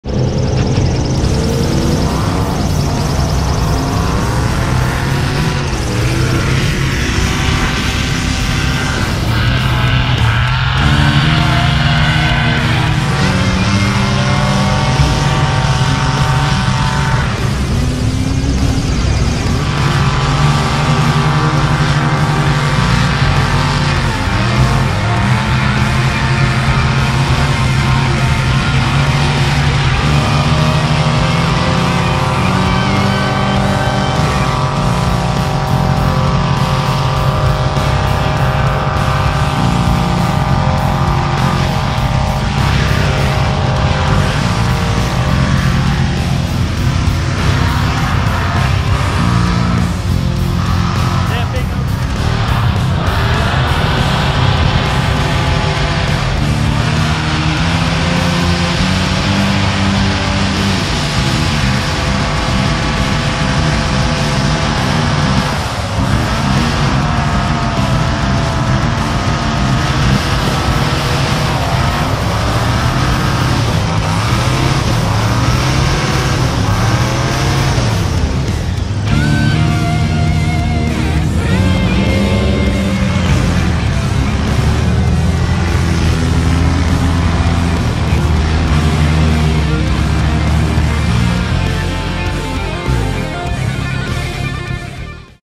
Florida Man Rides: When Too Much Is Not Enough.
One big boat is dragged threw the pasture by a even Bigger Boat.